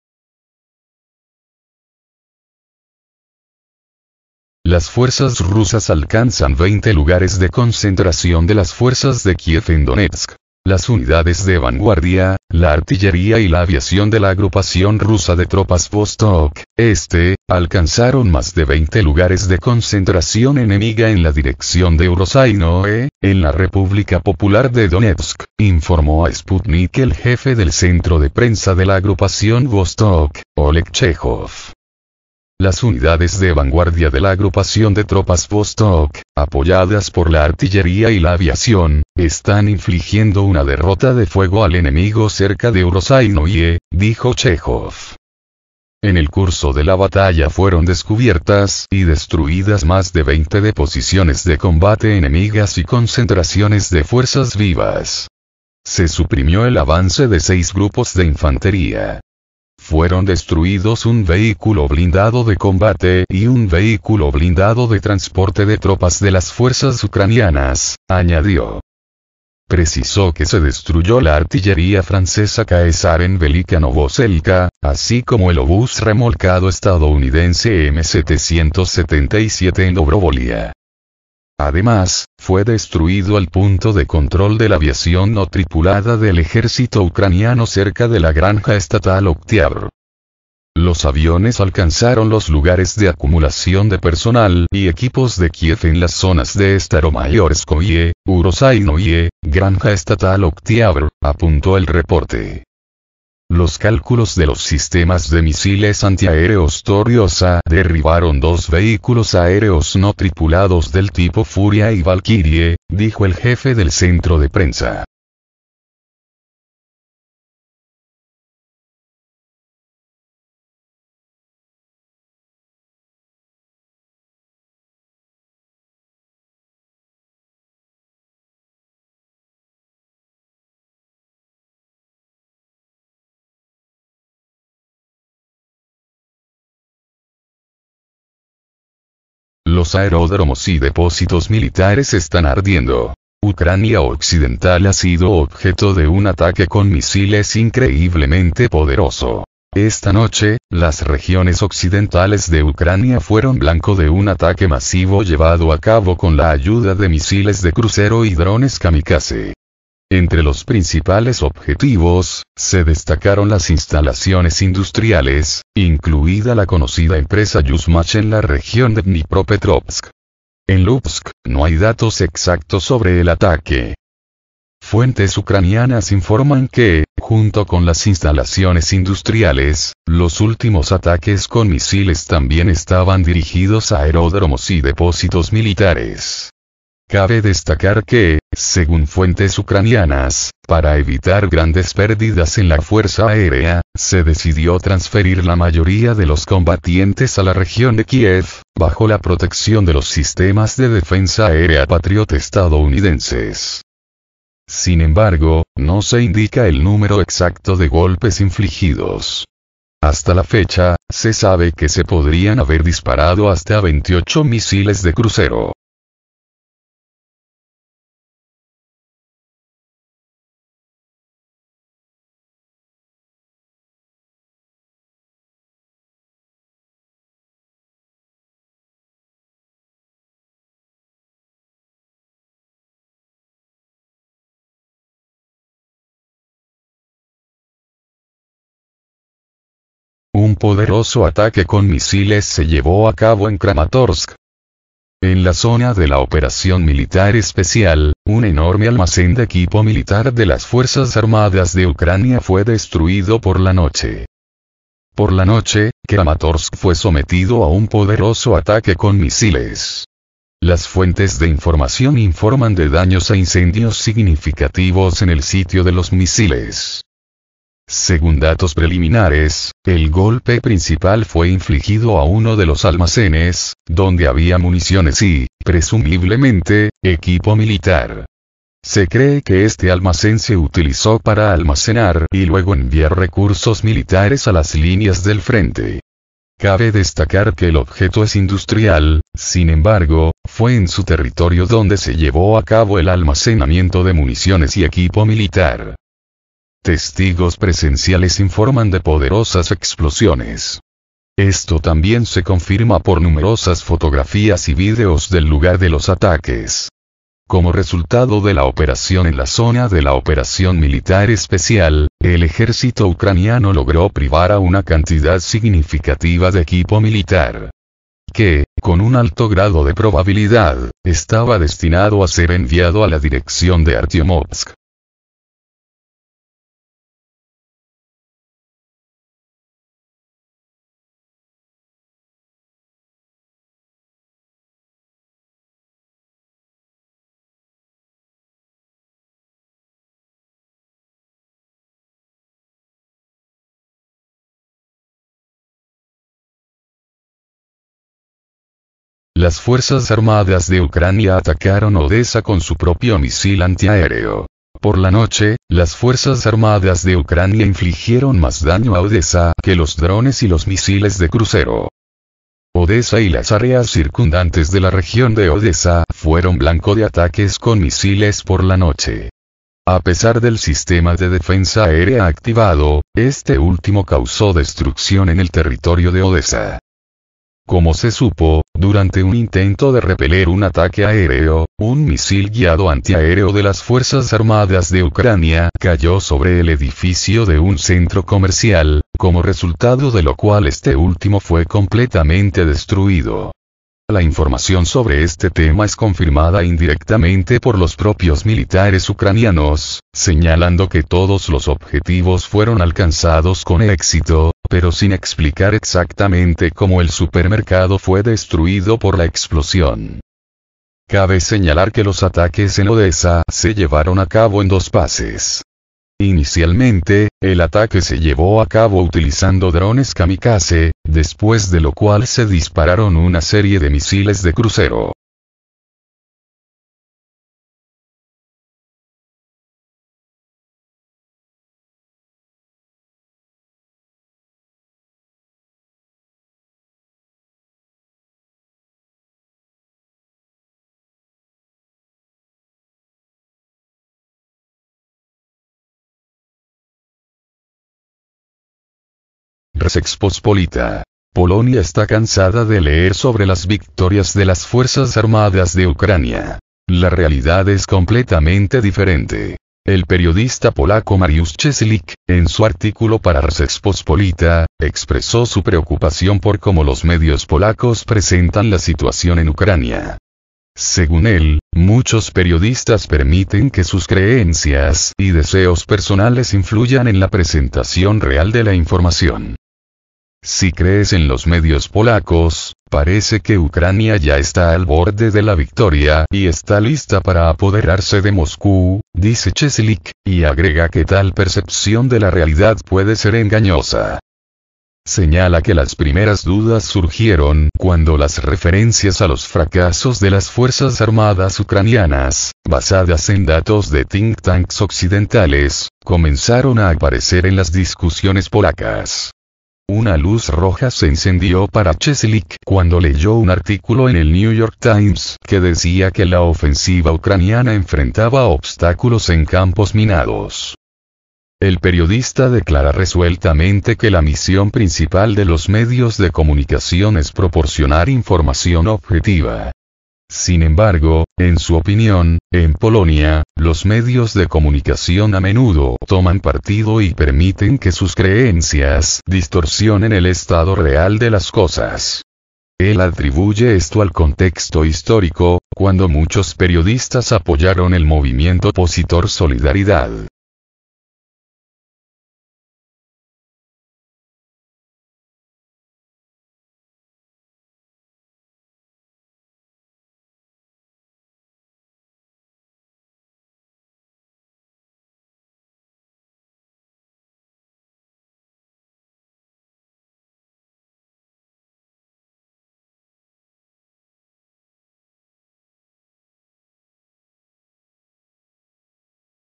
Destruidos 20 puntos de concentración de fuerzas de Kiev: Aeródromos y depósitos militares en llamas
Los aeródromos y depósitos militares están ardiendo: Ucrania occidental ha sido objeto de un ataque con misiles increíblemente poderoso

Un poderoso ataque con misiles se llevó a cabo en Kramatorsk.

Las Fuerzas Armadas de Ucrania atacaron Odessa con su propio misil antiaéreo.

Rzeczpospolita: Polonia está cansada de leer sobre las victorias de las Fuerzas Armadas de Ucrania; la realidad es completamente diferente